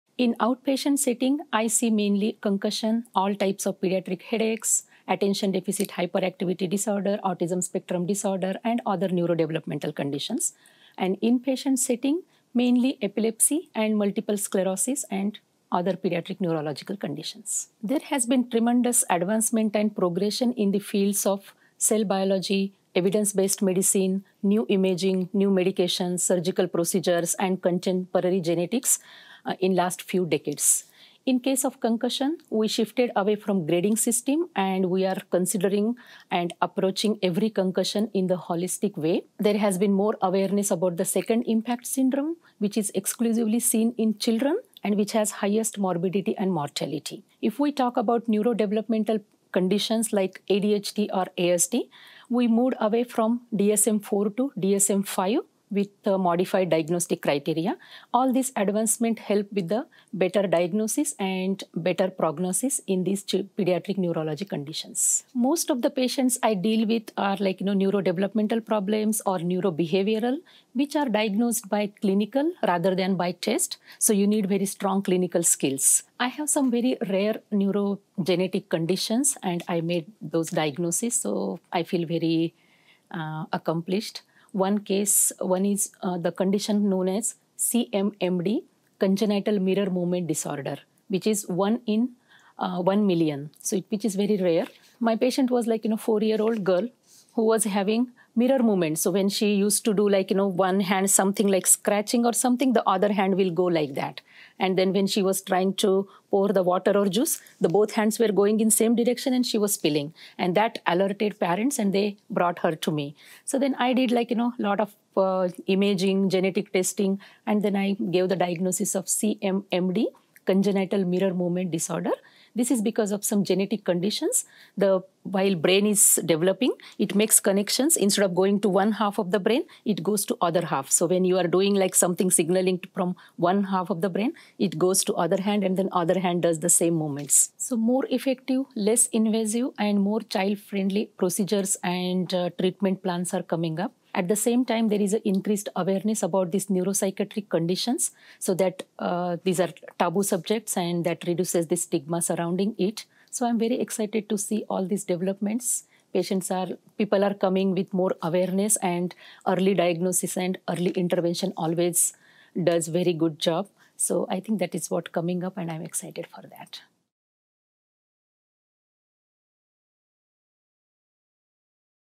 Dr. Bandari - Doximity
Dr. Savitra Bandari, M.D. is a pediatric neurologist. She has experience working with children and adolescents experiencing epilepsy, autism, concussions, headaches, neurodevelopmental conditions, behavioral conditions such as ADHD and hyperactivity, as well as rare neurogenetic disorders. Dr. Bandari takes a comprehensive approach to care, working with rehabilitation care partners and parents in areas of behavioral, occupational and physical therapy.